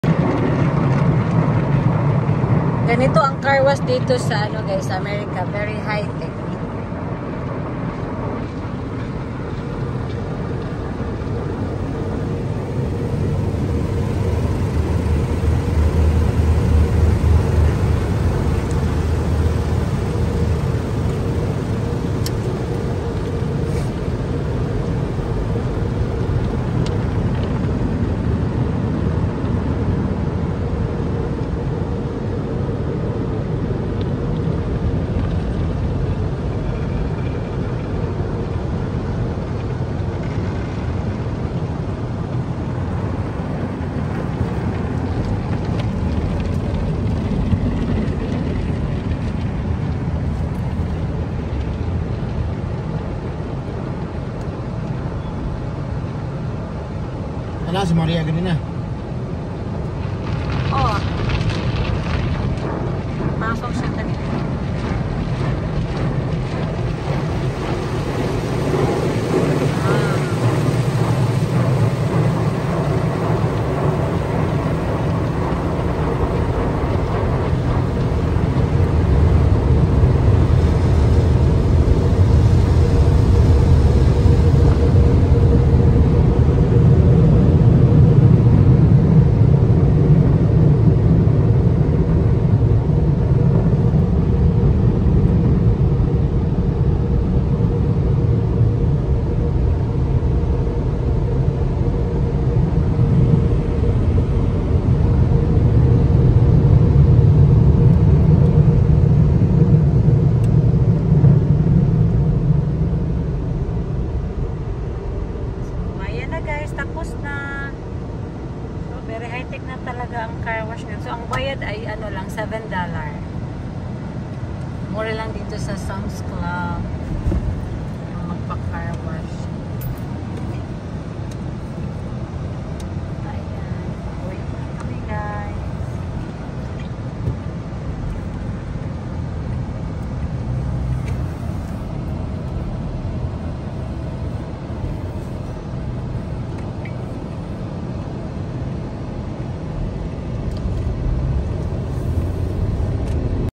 Carwash for only $7 /Samsclub Las Vegas /Melani Vlog
Very high tech na talga ngayon .Carwash day.